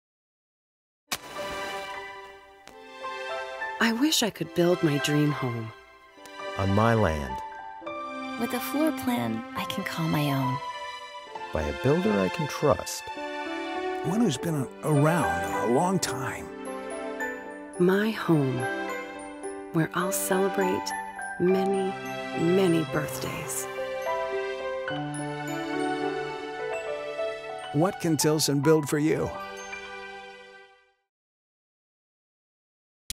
Tilson Homes Commercial - "I Wish"
Tilson Homes has been building homes for over 80 years. Tilson builds dream homes, on your land, and is trusted by more Texans. What can Tilson build for you?